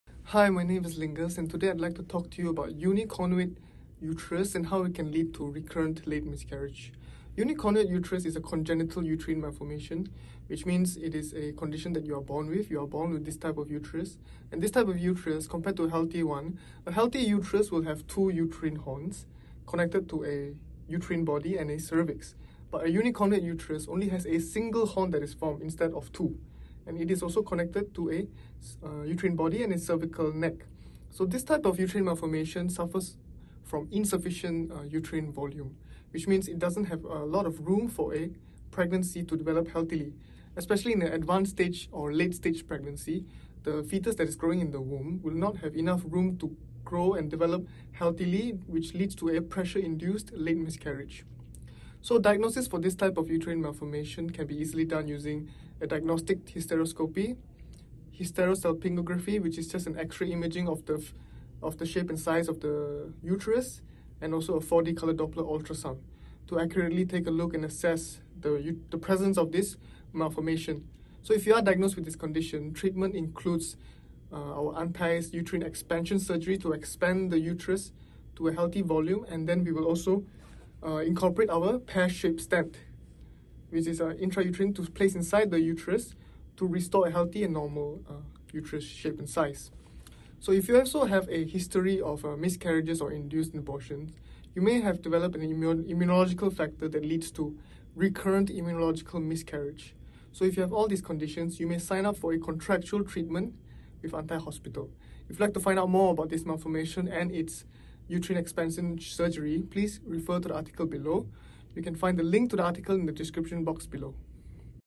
Unicornate Uterus & Late Miscarriage | Miscarriage Causes | Antai Hospital
Unicornuate uterus is a congenital abnormality that increases the risk of having a miscarriage.

A typical uterus is a muscular organ that looks like an inverted pear, where the fetus develops inside the top largest and hollowed portion of the uterus. A unicornuate uterus is a uterine malformation where only half of the uterus forms.

During pregnancy, the irregularly structured uterus may not expend fully to accommodate the growing fetus.  A late miscarriage is likely to occur due to space restriction in the uterus that obstructed the baby’s growth and development.

For patients with first-trimester miscarriage, click HERE to understand how a unicornuate uterus affected the pregnancy.

Note: Miscarriage and induced abortion may cause the development of Anti-embryonic Antibodies that will lead to recurrent early miscarriage.